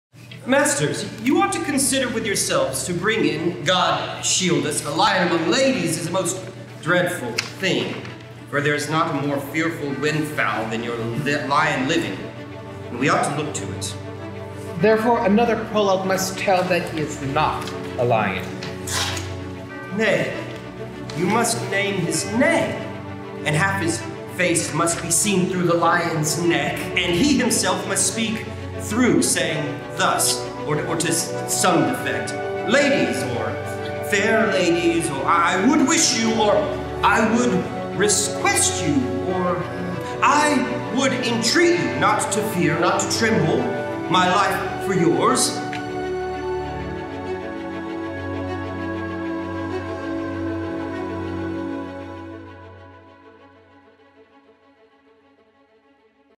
Midsummer 2020: An Immersive Experience — Coming Soon
Behind the scenes footage of table work on William Shakespeare's "A Midsummer Night's Dream" and of the production's first photo call.

Also functions as a test reel for the YI M1 mirrorless MFT camera.

Gear used:
- XIAOYI's YI-M1
- XIAOYI 42.5mm f/1.8 lens MFT mount
- Pentax 35-80mm f/4 lens K Mount
- JCPenney Optics 80-200mm f/5.5 lens K Mount
- Pentax 18mm f/2.8 lens Auto 110 mount
- Pentax 24mm f/2.8 lens Auto 110 mount
- Pentax 50mm f/2.8 lens Auto 110 mount
- iPhone 7
- Moment 58mm lens
- Moment 18mm lens
- Moment fisheye lens
- Tascam DR-40X
- Audio-Technica U851RO Boundary Mic